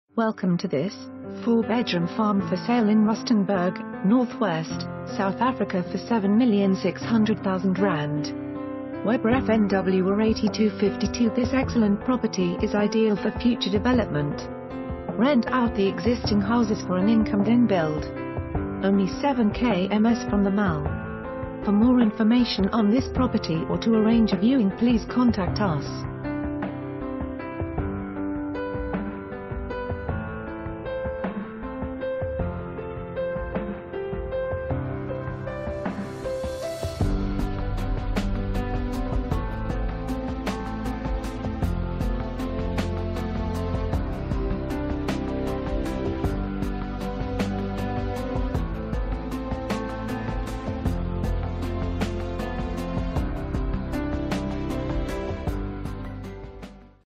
4 Bedroom Farm For Sale in Rustenburg Central, Rustenburg, North West, South Africa for ZAR 7,600...
Web Ref NWR8252: This excellent property is ideal for future development. Rent out the existing houses for an income then build. Only 7 kms from the Mall.